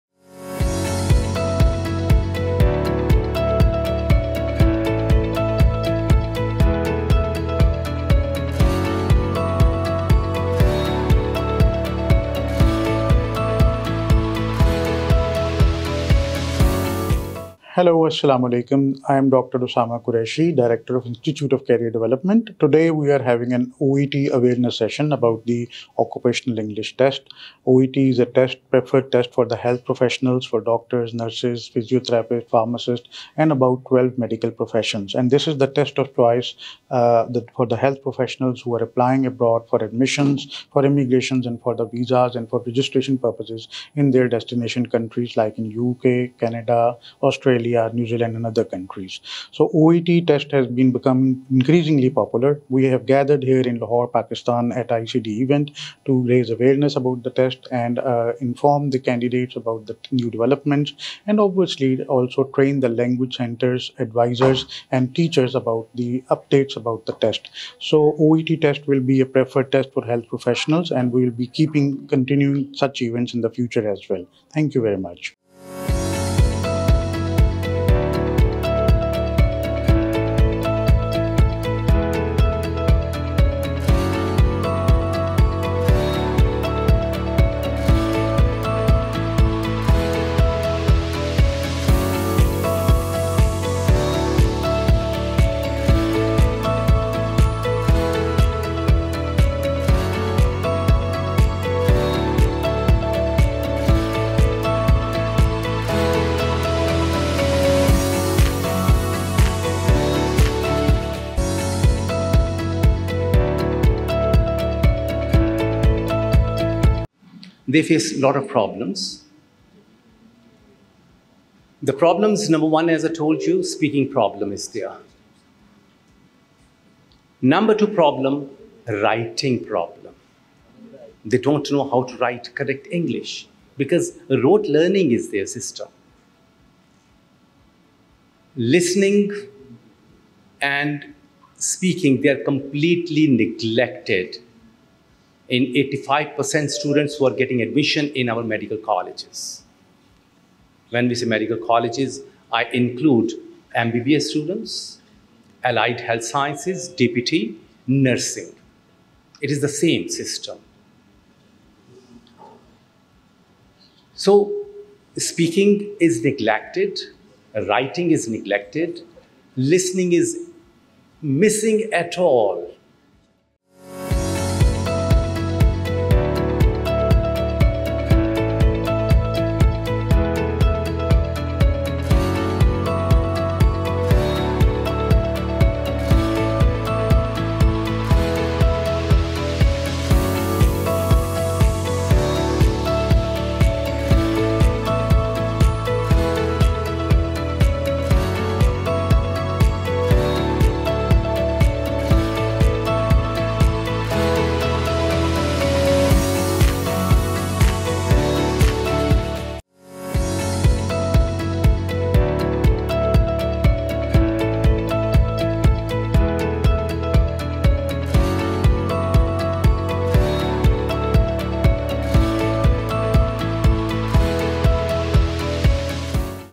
Occupation English Test (OET) Awareness Session for Healthcare Professionals
ICD in Pakistan organized an impactful OET awareness Session for healthcare professionals, featuring Tom Keenan Regional Director of OET, who provided insights into the OET exam through a live session.

Dr. Arif, Head of Department of the Medical and Dental College, graced us with his presence. Healthcare professionals, medical students, and teachers from various parts of Punjab joined us.

Special thanks to Lahore Rang Family the local news channel, for their coverage, which helped us reach a wider audience.